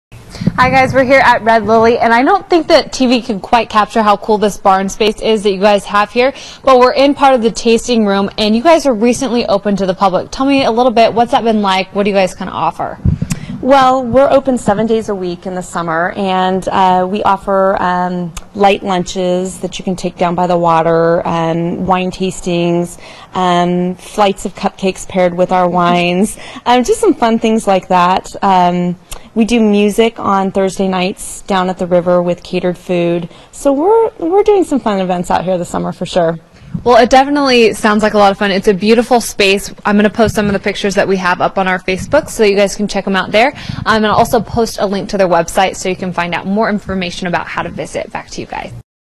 Cupcakes and wine
Red Lily Vineyard opens its tasting room seven days a week during the summer.They don't just serve wine. They do a cupcake and wine pairing as well as offer a light lunch.